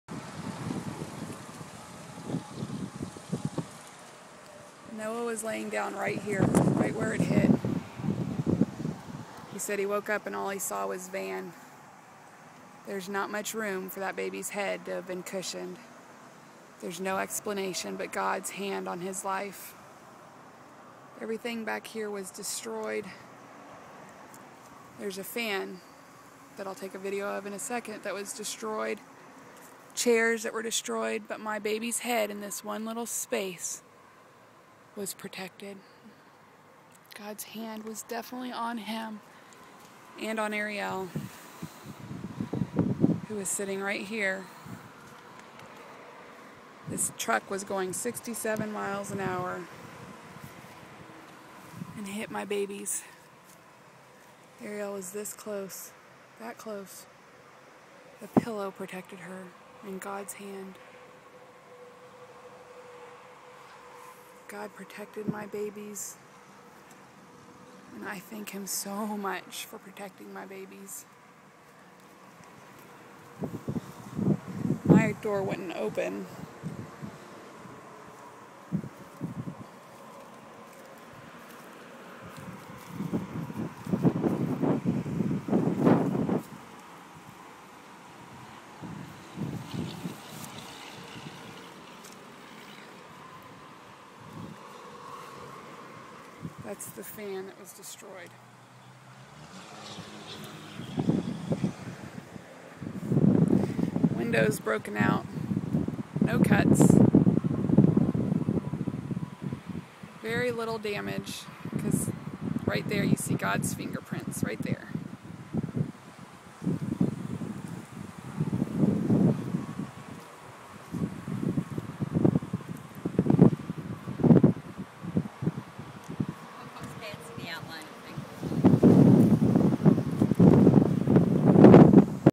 Don't drink and drive....
This is what our van looked like after being hit by a truck driven by a drunk driver, running a red light, and going almost 70 miles per hour.  My children are miracles to be alive, and definitely had angels protecting them that night! I was life flighted to the hospital and healed miraculously through prayer (thank you, Jesus!!).  I was life flighted in and discharged out within a few hours.  Nothing but a miracle can explain many things that have come out of this accident.  I don't wish it upon anyone, but thank God for allowing us to see so many of His amazing ways through it all!

Please be respectful in your comments or they will be deleted.